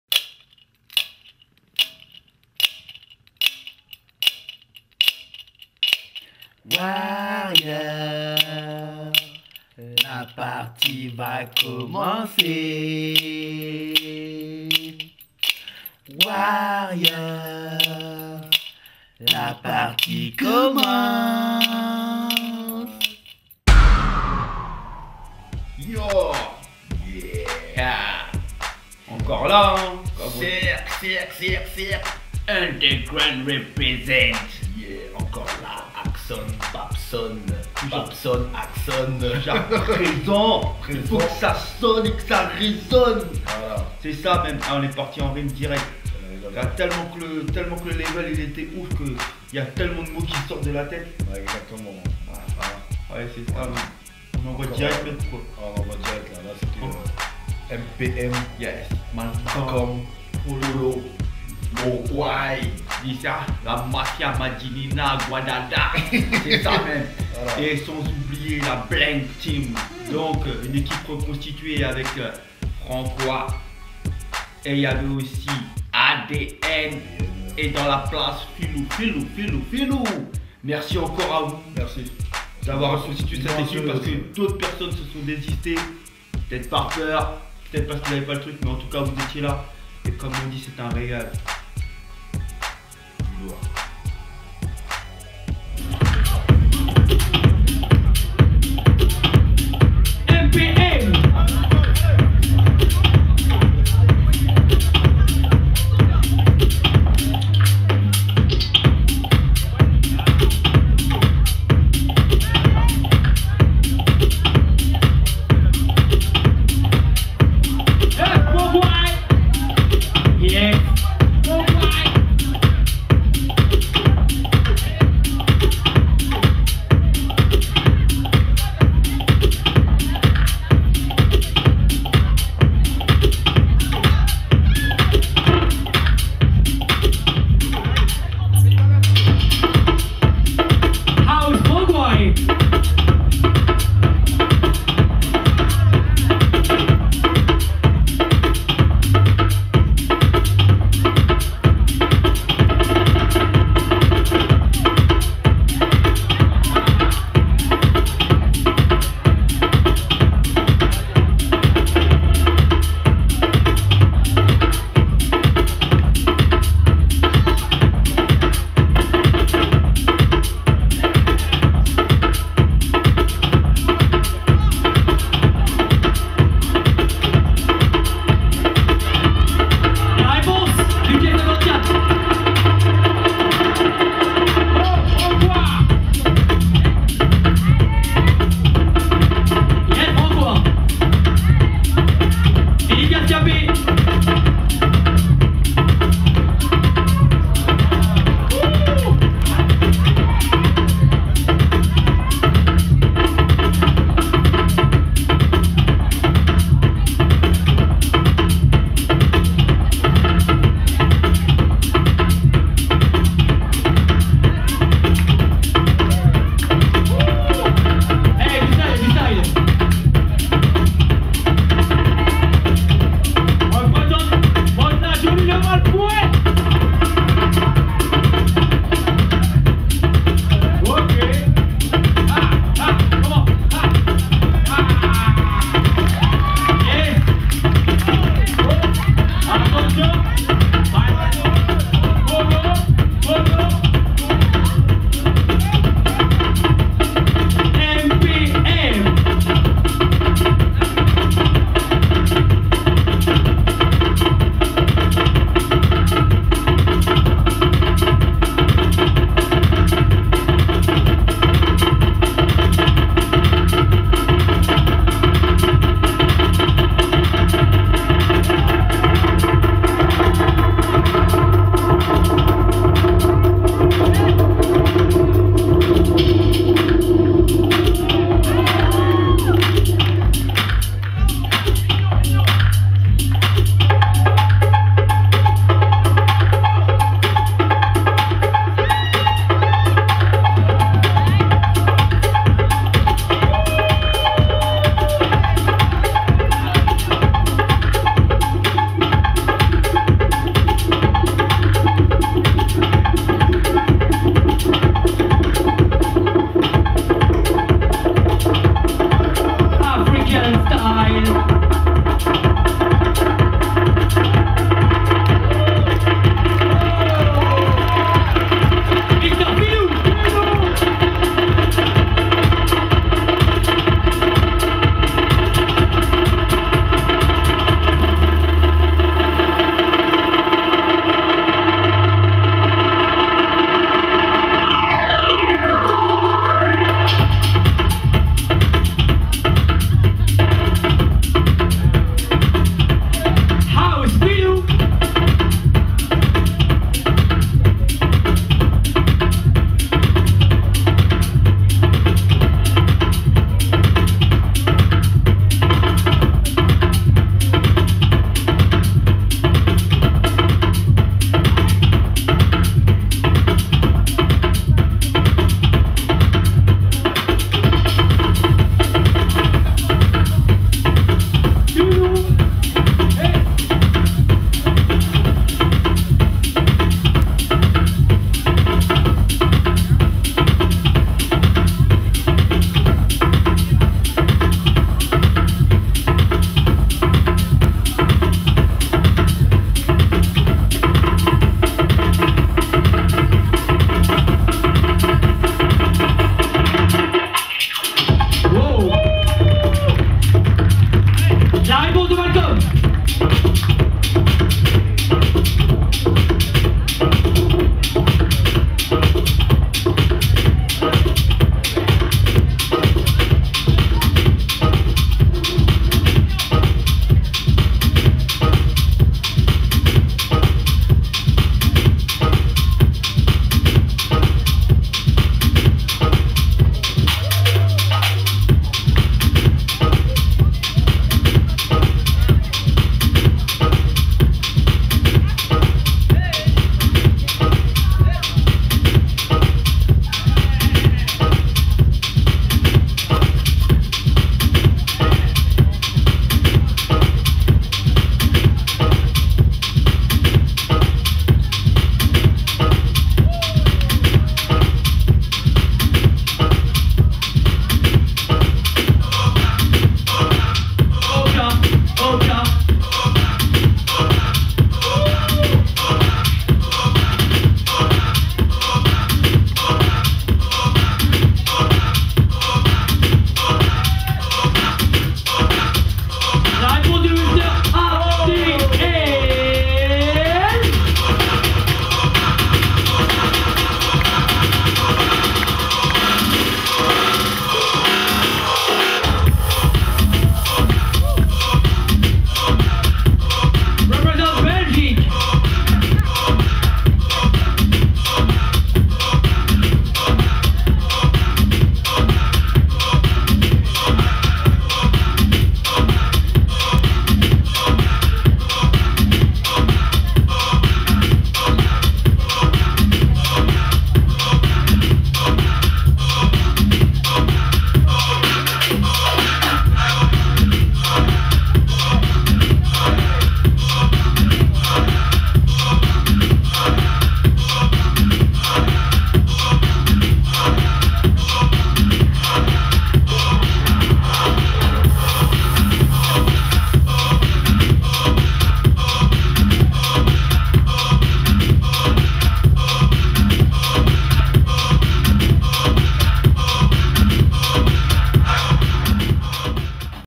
Cercle Underground House 1/4 final MPM Vs Blank Team 1st part
Battle Cercle Underground 1ère Edition organisé par Hagson Dimanche 18 Avril 2010.
1/4 finale House MPM Vs Blank Team 1ère partie .
Speakers : Youval & Babson
Dj's : Yugson & Someone
Captation Realisation Montage : Karlos "leKameleon" da Silva (Karism Pictures)